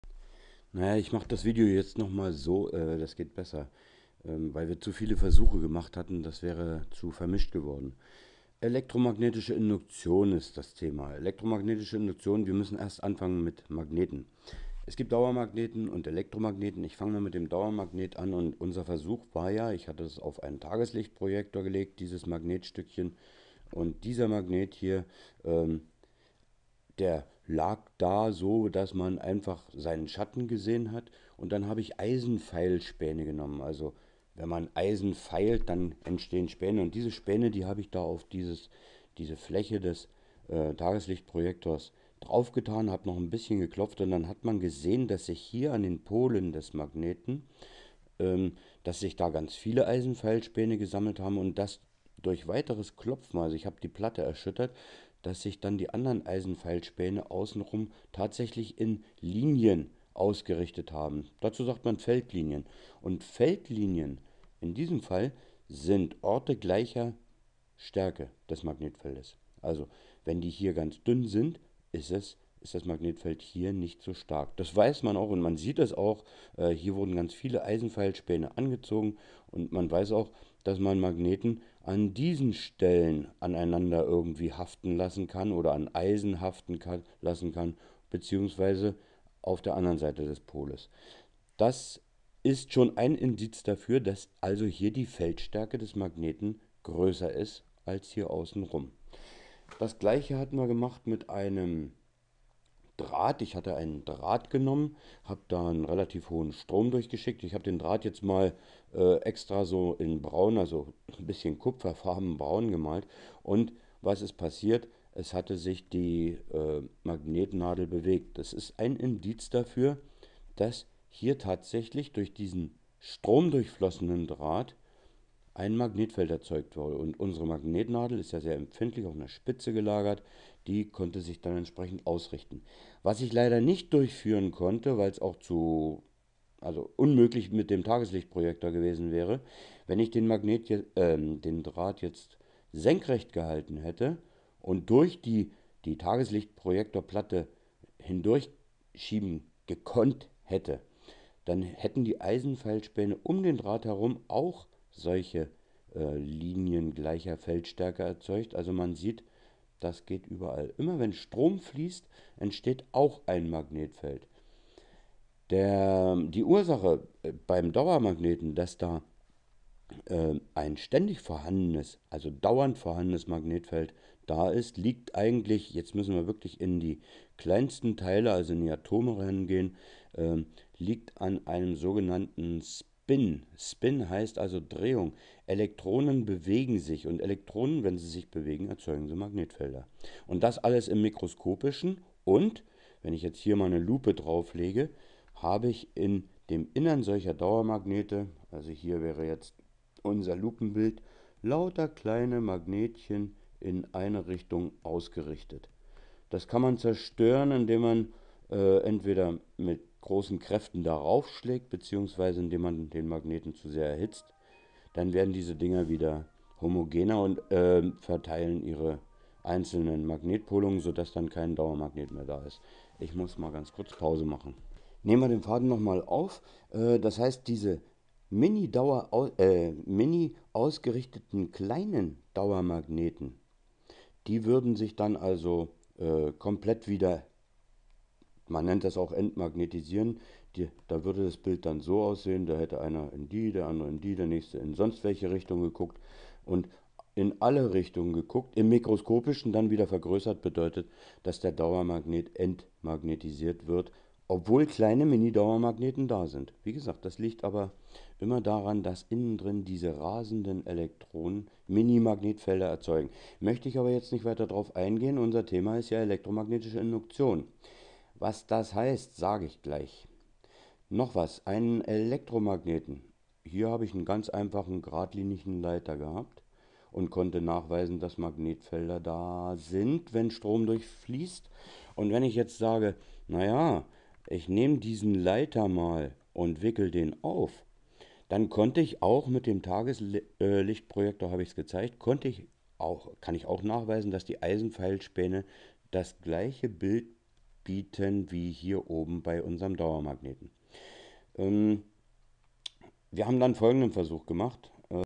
Elektromagnetische Induktion, Teil 0a, WICHTIG, zuerst Erklärung Permanentmagnet, Elektromagnet, ...
Bitte, helfen Sie uns Endlich wieder (Sapere aude!) ... Feldlinien der Magnete, Wechselwirkung: Strom zu Magnetfeld und umgekehrt. Entmagnetisieren eines Dauermagneten usw. Lieber verstehen als auswendig lernen. (Björn Köhler, MaPhy)

09 2014_09_12 :zzzzz_intern Aufgaben